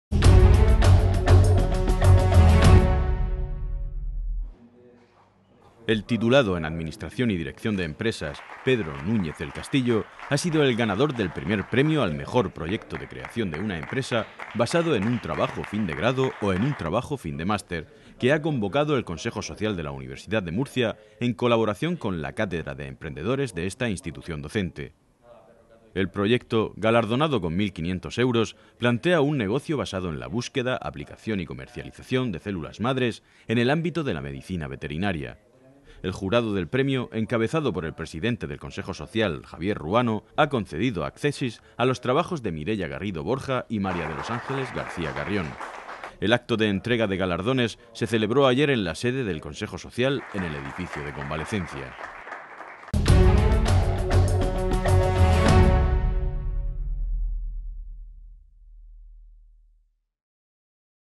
I Premio Creación Empresa del Consejo Social
El titulado en ADE Pedro Núñez del Castillo ha sido el ganador del I Premio al Mejor Proyecto de Creación de una Empresa basado en un Trabajo Fin de Grado o en un Trabajo Fin de Máster, que ha convocado el Consejo Social de la Universidad de Murcia en colaboración con la Cátedra de Emprendedores de esta institución docente.

El proyecto, galardonado con 1.500 euros, plantea un negocio basado en la búsqueda, aplicación y comercialización de células madres en el ámbito de la medicina veterinaria.

El jurado del premio, encabezado por el presidente del Consejo Social, Javier Ruano, ha concedido accésits a los trabajos de Mireya Garrido Borja y María de los Ángeles García Carrión.

El acto de entrega de galardones se celebró ayer en la sede del Consejo Social, en el edificio de Convalecencia.